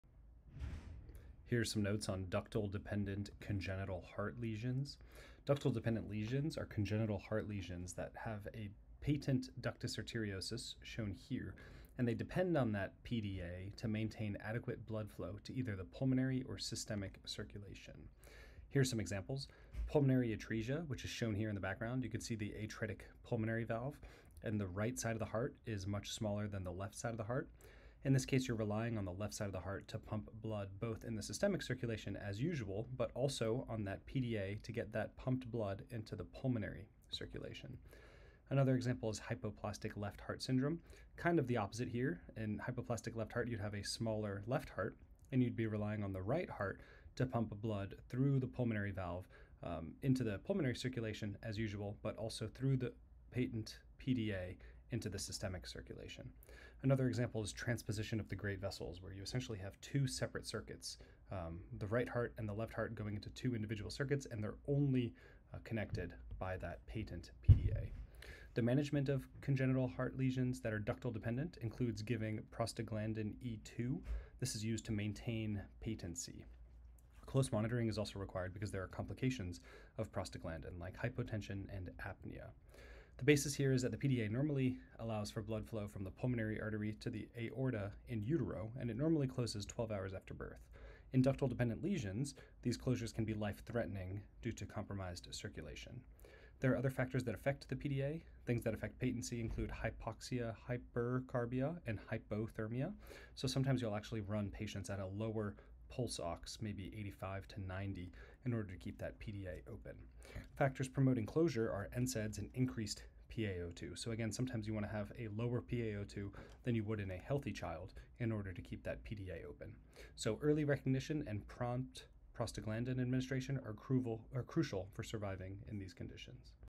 Ductal dependent congenital heart lesions
Ductal dependent lesions are congenital heart defects that require a patent ductus arteriosus (PDA) to maintain adequate blood flow to either the pulmonary or systemic circulation.

1. Common examples include:
   - Pulmonary atresia
   - Hypoplastic left heart
   - Transposition of the great vessels

2. Management:
   - Prostaglandin E2 (PGE2) is used to maintain PDA patency
   - Close monitoring required due to potential complications of PGE2:
     - Hypotension
     - Apnea

3. Physiological basis:
   - PDA normally allows blood flow from pulmonary artery to aorta in utero
   - Normally closes within 12 hours after birth
   - In ductal dependent lesions, closure can be life-threatening due to compromised circulation

4. Other factors affecting PDA:
   - Factors promoting patency: hypoxia, hypercarbia, hypothermia
   - Factors promoting closure: NSAIDs, increased PaO2

Early recognition and prompt PGE2 administration are crucial for survival in these conditions.